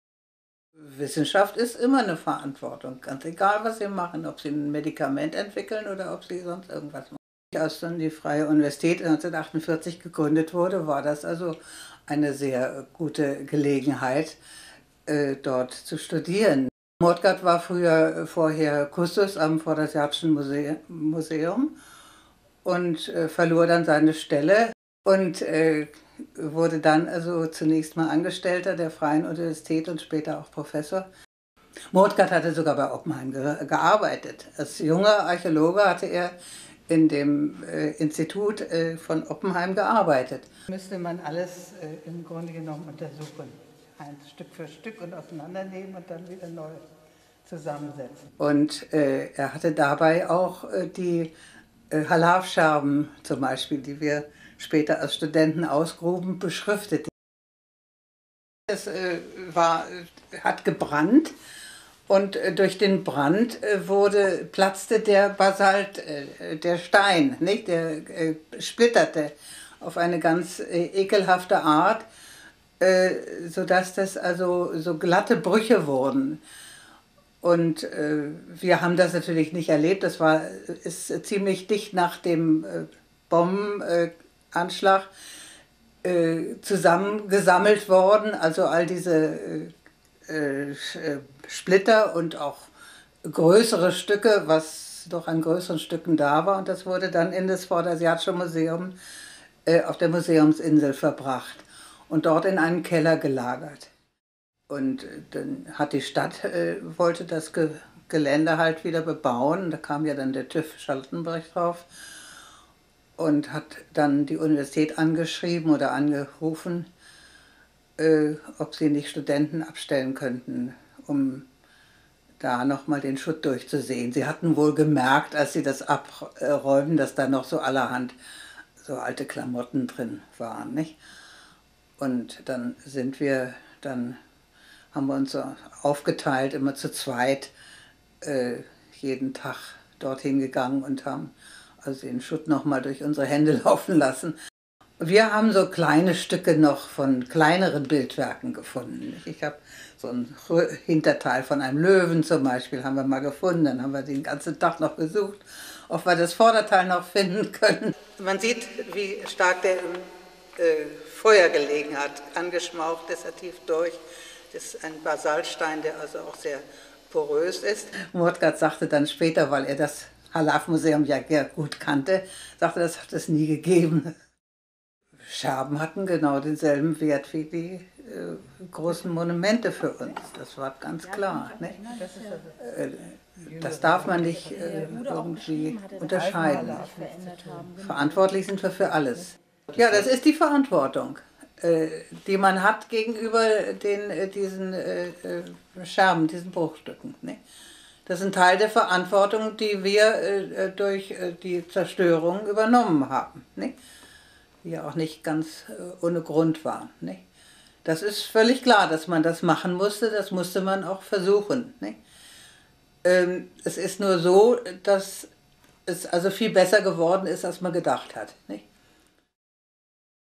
Die geretteten Götter aus dem Palast vom Tell-Halaf | Interview mit Eva Strommenger
Eva Strommenger gehörte zu den ersten Studierenden des Faches "Vorderasiatische Altertumskunde" an der Freien Universität Berlin. Mit ihren Kommilitonen suchte sie 1951 im Auftrag von Professor Anton Moortgat auf dem Ruinengelände nach weiteren Überresten. Im Interview
erzählt sie von dieser zweiten Bergungsaktion.